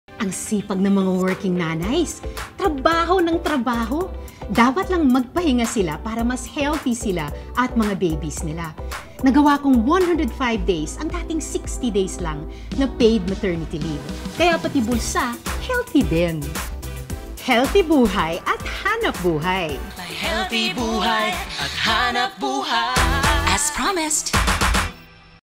Para sa mga Working Nanays HealthyBuhay at HanapBuhay | Sen. Risa Hontiveros
Saludo talaga si Risa Hontiveros sa sipag ng working moms kaya deserve nila ang Expanded Maternity Leave na mula 60 days ay naging 105 ang paid maternity leave para maalagaan si baby nang maayos.